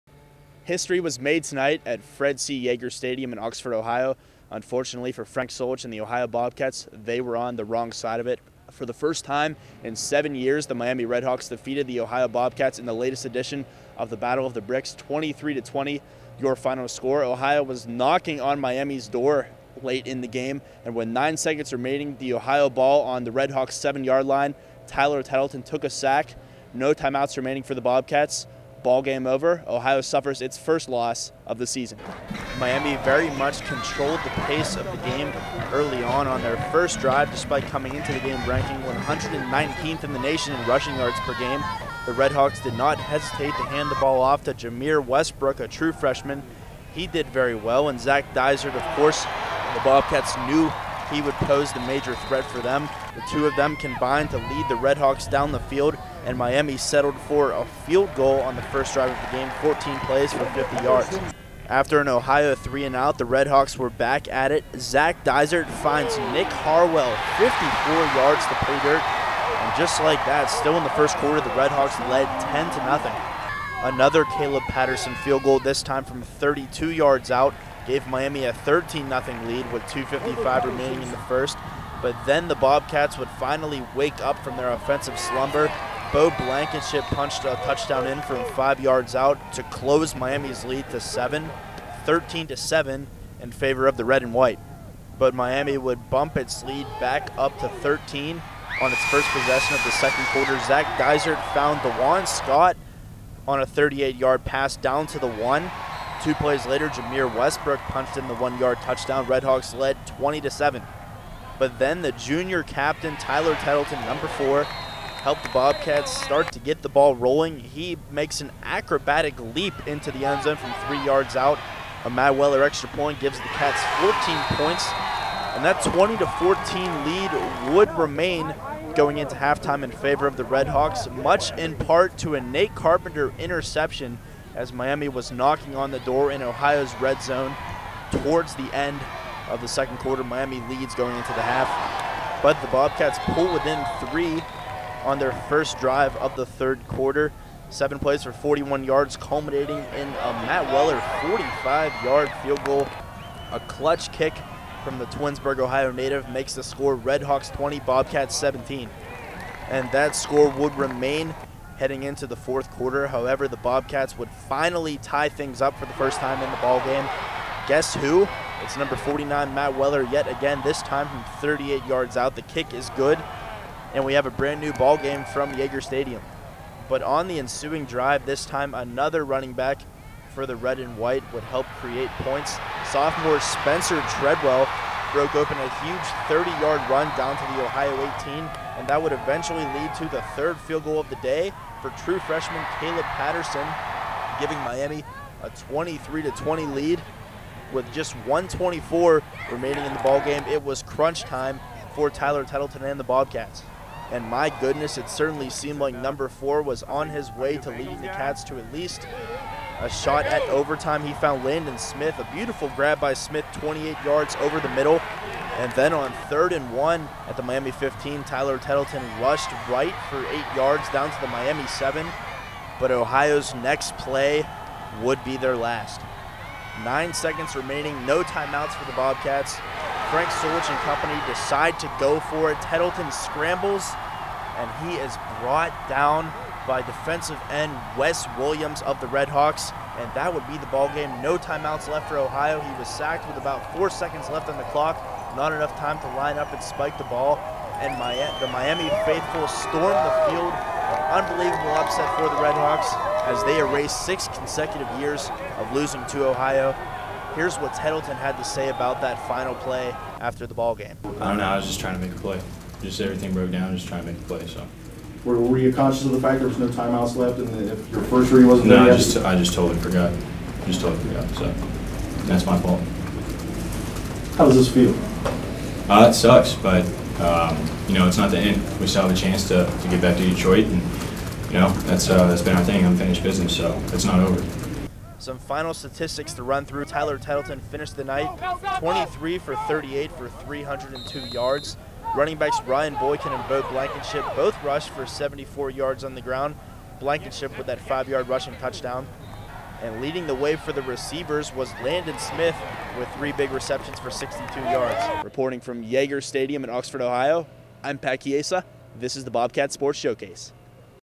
Video Recap: Miami 23, Ohio 20 (10/27/12)
The Miami RedHawks defeated the Ohio Bobcats for the first time since 2005 on Saturday, kicking a field goal late in the fourth quarter to steal a 23-20 victory from the previously unbeaten 'Cats. Ohio will look to rebound this Thursday as the Eastern Michigan Eagles come to town looking for an upset.